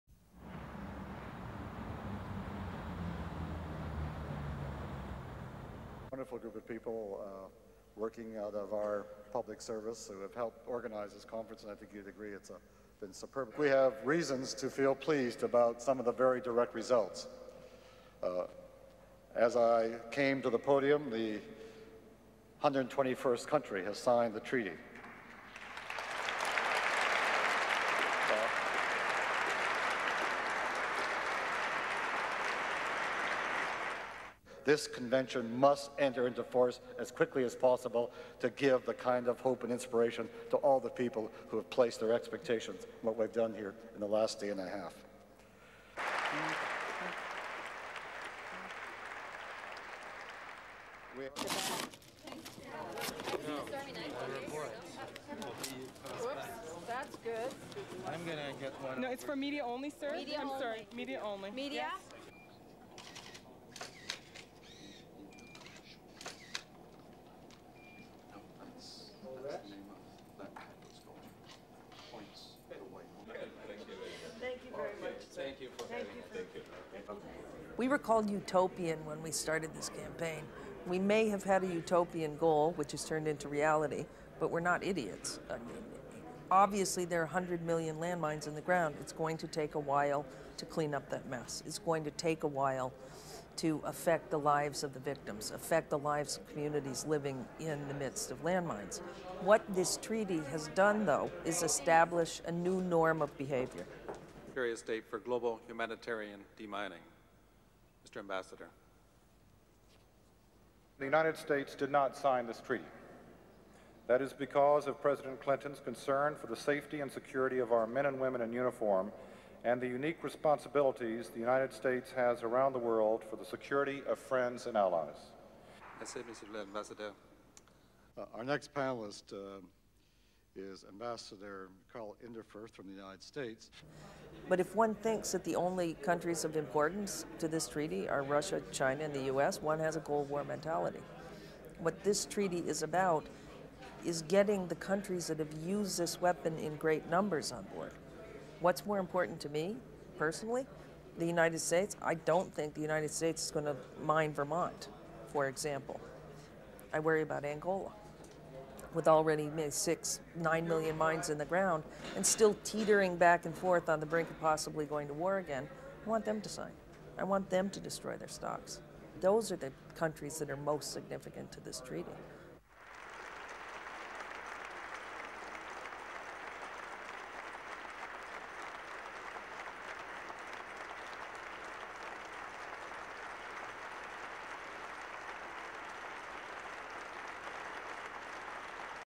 CANADA: OTTAWA: ANTI LANDMINE TREATY CONFERENCE CLOSING SESSION
Delegates who made history by signing a treaty banning landmines ended the Ottawa  conference on Thursday with pledges to help fund mine-clearance programmes.

But the Canadian foreign minister, a key figure in rallying support for the ban, said during the closing session of the conference that the biggest challenges were still ahead.

These include raising hundreds of millions of dollars to help poor nations remove mines, ensuring that the treaty is enforced and convincing major holdouts - such as China, Russia and the United States - to join the 121 nations that have endorsed the plan.

In the frozen Canadian capital, the will of an international coalition of governments, humanitarian organisations and activists made history this week.

A global treaty to ban and destroy anti-personnel landmines was endorsed by more than half the nations on Earth.

SOUNDBITE (English)
"We have reasons to feel pleased about some of the very direct results. As I came to the podium the one hundred and twenty first country has signed the treaty."
SUPER CAPTION: Lloyd Axworthy, Canadian Foreign Minister

The ban calls on countries to destroy their own stockpiles in four years and dig up any mines they have laid within a decade.

But while the mood was heady, the conference host warned against complacency.

SOUNDBITE (English)
"This convention must enter into force as quickly as possible to give the kind of hope and inspiration to all the people who have placed their expectations in what we've done here in the last day and a half."

Along with the surprising support for the principles of the treaty, many nations - even those who refused to sign the ban - are pledging millions (m) to dig up and destroy mines and to care for those maimed by them.

Axworthy told delegates that half a billion (b) U-S dollars had been pledged to help implement the treaty.

The conference produced a comprehensive agenda for future action.

It detailed schedules and timetables sure to keep governments, non-governmental organisations and activists hopping until 40 nations ratify the treaty and codify it into international law.

While more than 120 nations signed on to the treaty - something unimaginable a year ago - several key holdouts threatened to suck the credibility out of the pact.

But Nobel Prize-winning peace activist Jody Williams said she had no illusions about the treaty and what could be accomplished, politically and logistically.

SOUNDBITE (English)
"We were called Utopian when we started this campaign. We may have had a Utopian goal, which has turned into reality, but we're not idiots. Obviously there are 100 million (m) landmines in the ground. It's going to take a while to clean up that mess. It's going to take a while to affect the lives of the victims, affect the lives of communities living in the midst of landmines. What this treaty has done though is establish a new norm of behaviour."
SUPER CAPTION: Jody Williams, Nobel Peace Prize Winner

The United States sought an exemption for the Korean peninsula, but the treaty organisers held to their principals, painting the U-S into a corner.

SOUNDBITE (English)
"The United States did not sign this treaty. That is because of President Clinton's concern for the safety and security of our men and women in uniform and the unique responsibilities the United States has around the world for the security of friends and allies."
SUPER CAPTION: Karl Inderfurth, U-S observer

China and Russia also refused to sign, as did Israel and Lebanon.